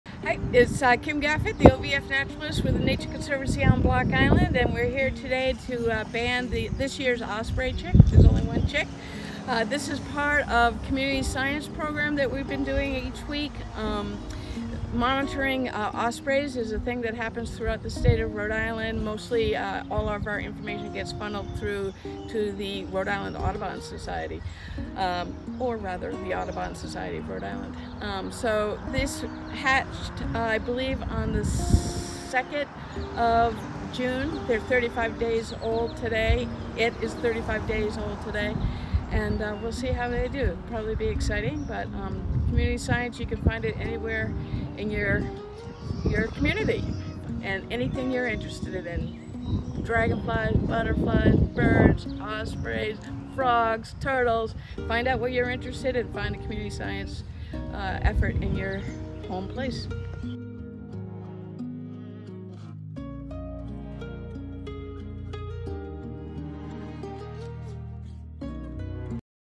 Community Science: Osprey Chick on Block Island
Monitoring an Osprey nest is one example of a community science project that anyone can do. Find something that you're interested in -- frogs, dragonflies, birds, etc -- and no doubt, there's a community science effort on that topic in your home place.